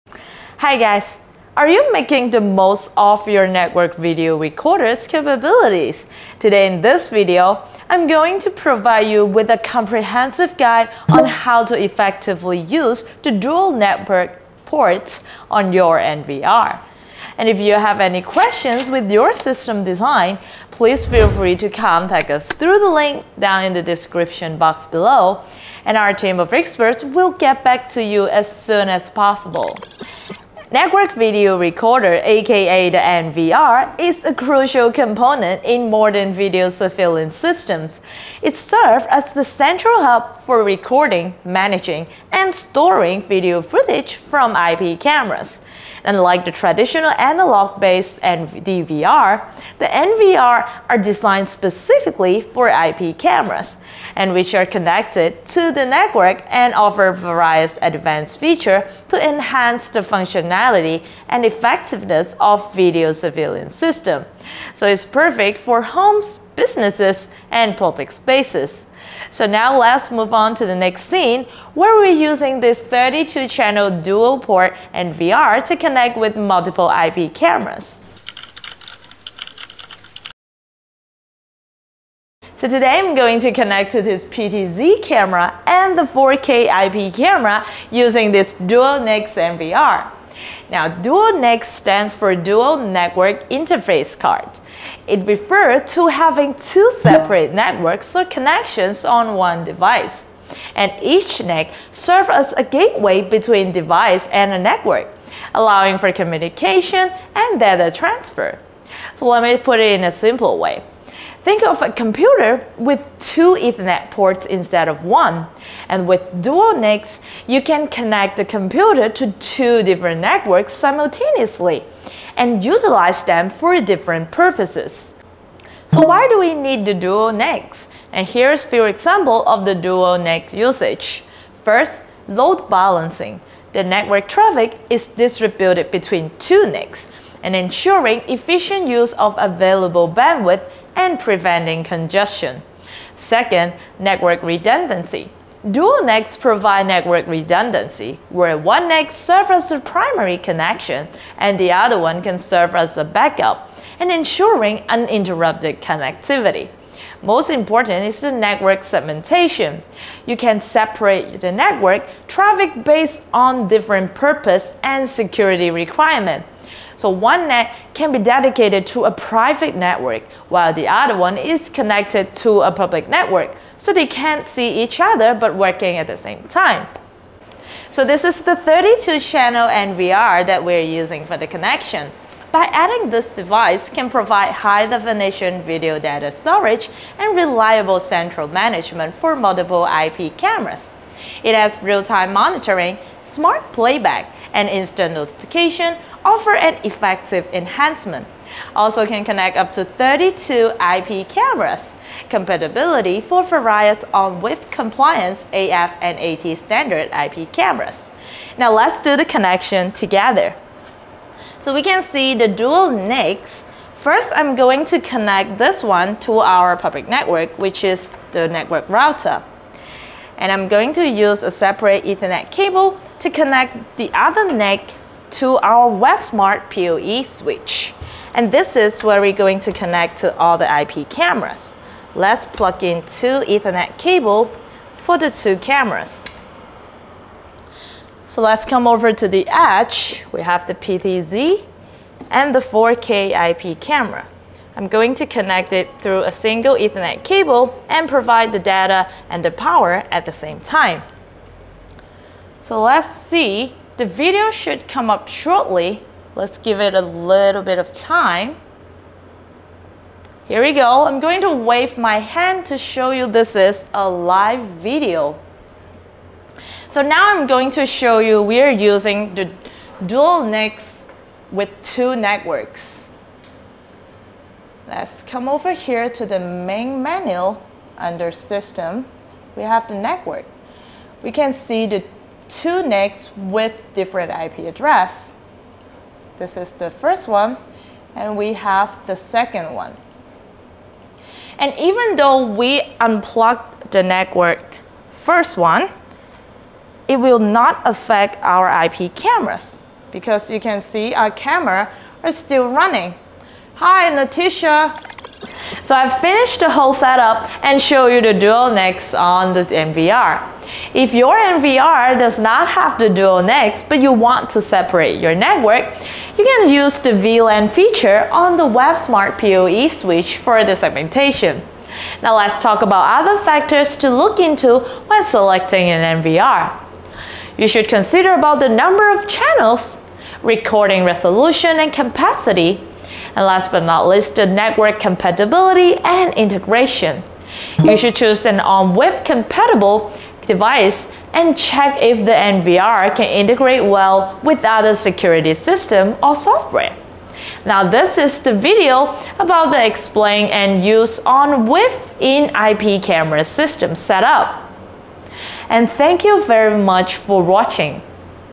Two Network Ports on One NVR | How to Use Dual NICS
In this video, we will explore the concept of using dual NICs (Network Interface Cards) in a Network Video Recorder (NVR) for enhanced network connectivity and data redundancy. You will have a clear understanding of how to leverage dual NICs to separate your surveillance network and enjoy the benefits of improved security and optimized network performance.

0:00:00 Intro
Brief Introduction to NVR
Types of NVR
What is DUAL NICs NVR?
How to Use Dual NICS on NVR for Separating Networks?
Factors to Look into When Selecting NVR
Onvif Compatibility Explained